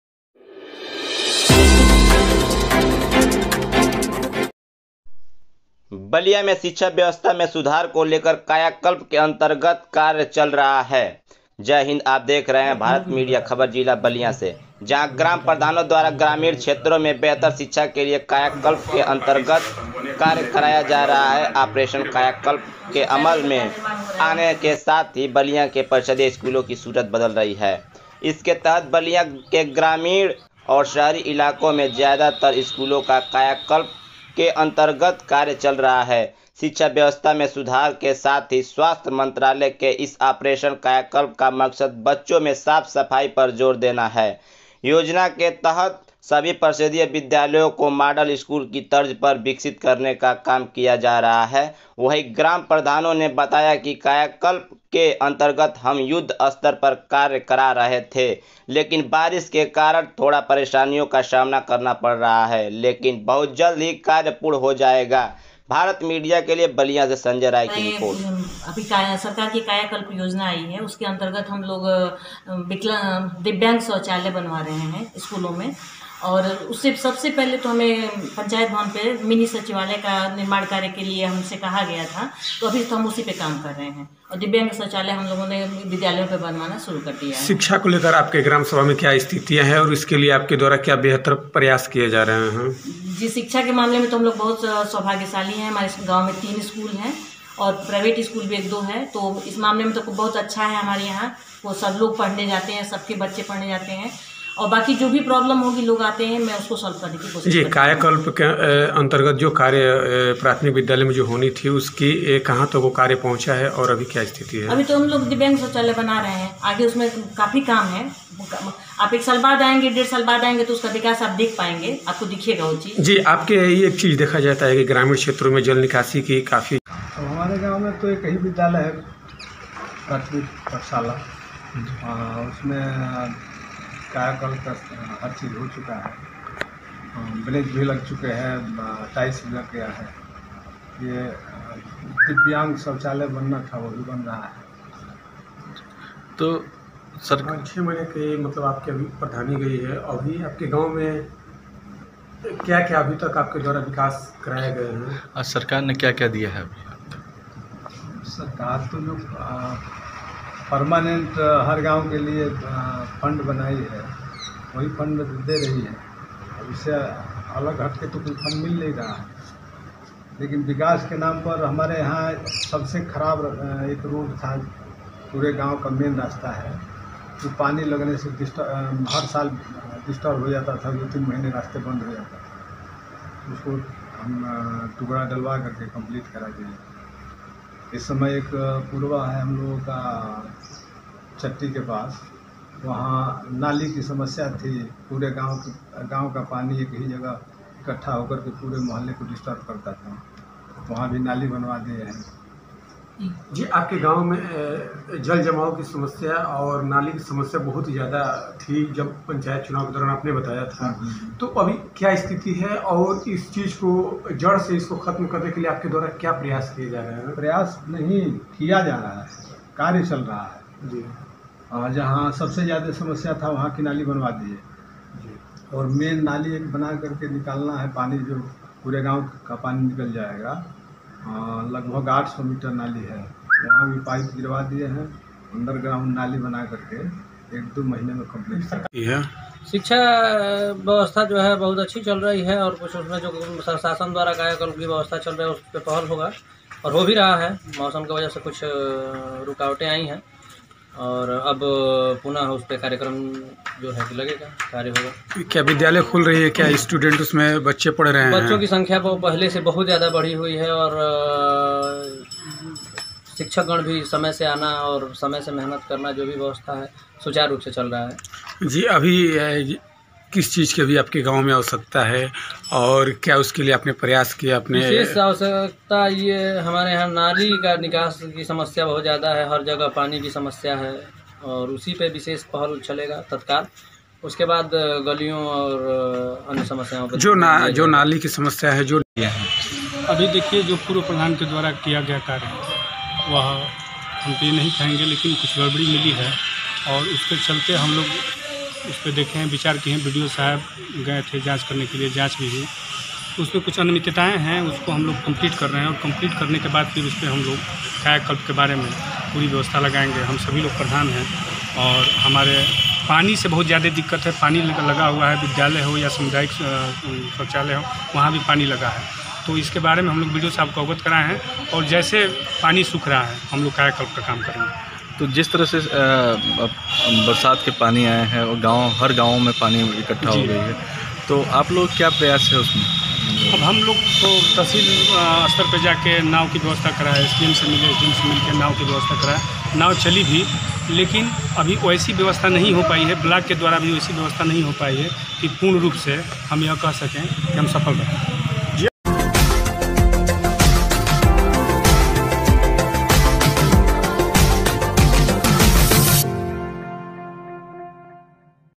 Ballia || शिक्षा व्यवस्था में सुधार को लेकर कायाकल्प के अंतर्गत कार्य चल रहा
बलिया में शिक्षा व्यवस्था में सुधार को लेकर कायाकल्प के अंतर्गत कार्य चल रहा है।

 *एंकर-* खबर यूपी के बलिया से है। जहां ग्राम प्रधानों द्वारा ग्रामीण क्षेत्रों में बेहतर शिक्षा के लिए कायकल्प के अंतर्गत कार्य कराया जा रहा है। आपरेशन कायाकल्प के अमल में आने के साथ ही बलिया के परिषदीय स्कूलों की सूरत बदल रही है। इसके तहत बलिया के ग्रामीण और शहरी इलाकों में ज्यादा तर स्कूलों का कायाकल्प के अंतर्गत कार्य चल रहा है। शिक्षा व्यवस्था में सुधार के साथ ही स्वास्थ्य मंत्रालय के इस ऑपरेशन कायाकल्प का मकसद बच्चों में साफ-सफाई पर जोर देना है। योजना के तहत सभी परिषदीय विद्यालयों को मॉडल स्कूल की तर्ज पर विकसित करने का काम किया जा रहा है। वही ग्राम प्रधानों ने बताया कि कायाकल्प के अंतर्गत हम युद्ध स्तर पर कार्य करा रहे थे। लेकिन बारिश के कारण थोड़ा परेशानियों का सामना करना पड़ रहा है। लेकिन बहुत जल्द ही कार्य पूण हो जाएगा।

 *बाइट- सुमन मिश्र*
( *ग्रामप्रधान सोनवानी), कामेश्वर* *यादव(ग्रामप्रधान सरहसपाली),नितिन* *पांडेय(ग्रामप्रधान पिण्डहरा), जितेन्द्र* *कुमार( ग्राम प्रधान)*
भारत media